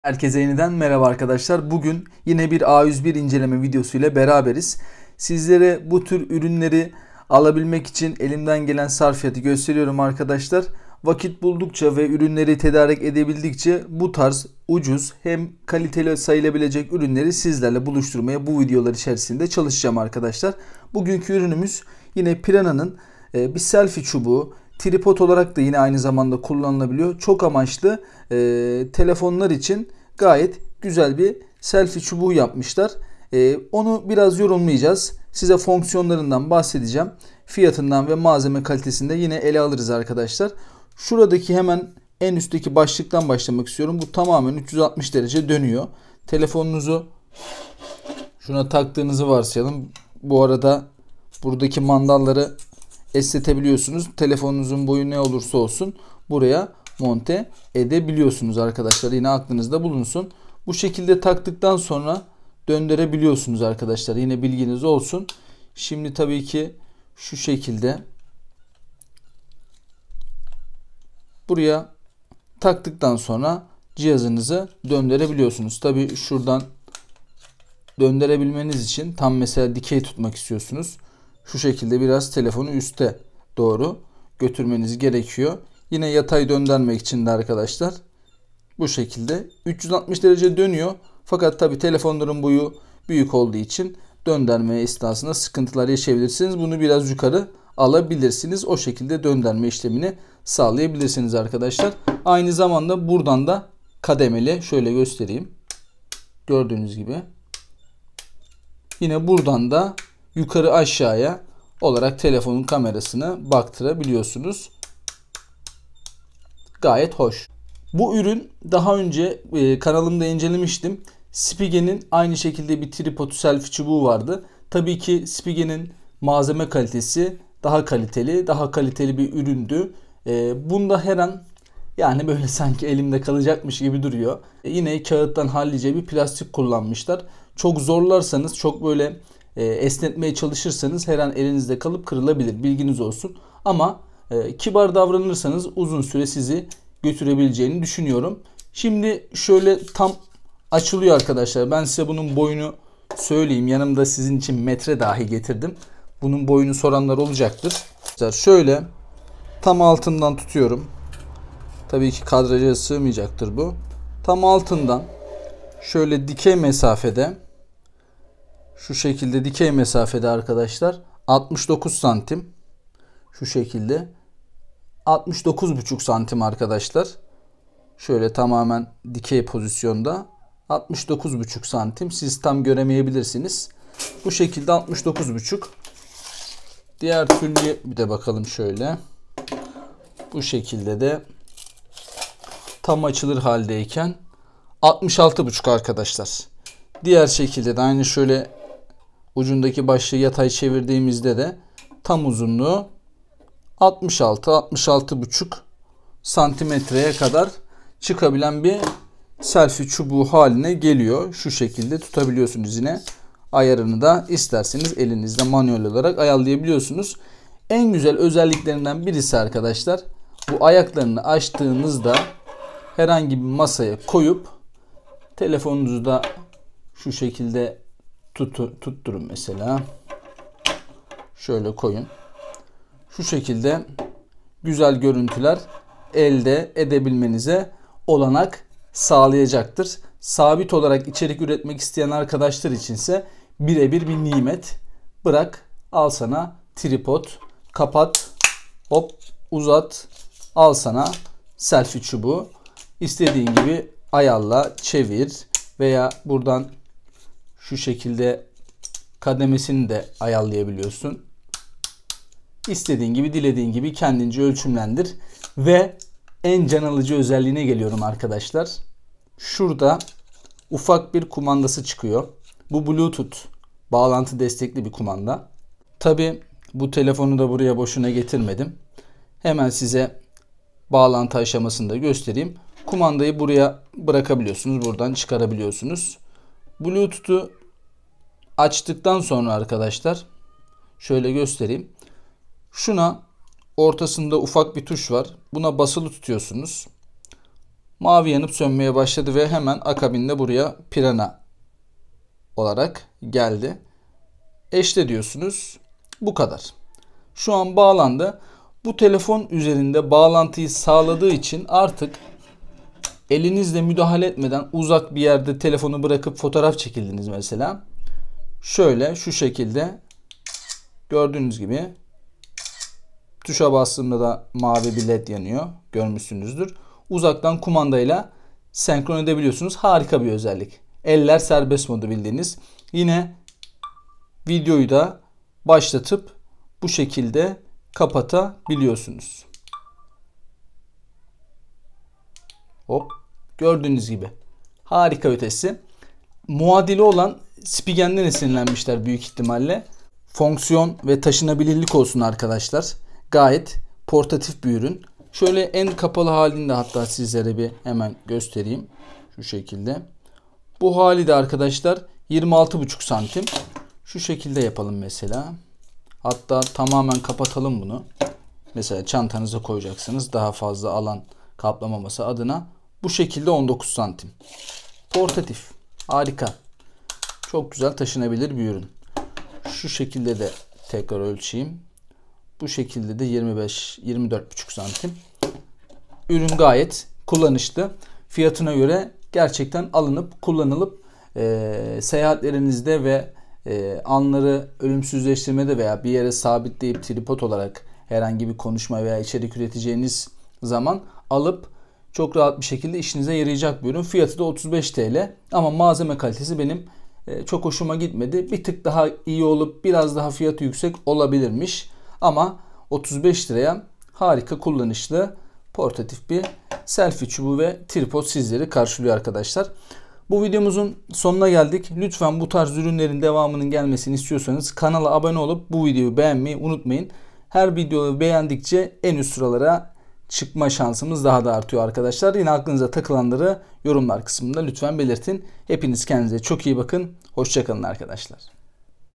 Bedava FİYATINA ! Piranha Uzaktan Kumandalı Tripod ve Selfie Çubuğu
Merhaba Arkadaşlar,

A101'den satın aldığımız Piranha marka uzaktan kumandalı,tripod ve selfie çubuğu incelemesi yapacağız. Kurulum ve malzeme kalitesi,kullanılabilirlik gibi kriterleri baz alarak incelemeye çalıştık. Fiyatına göre rakipsiz olduğunu söyleyebilirim :) Detaylar video içerisinde ! Herkese iyi seyiler.

adresimiz üzerinden güncel teknoloji haberlerini ve trendlerini takip edebilirsiniz.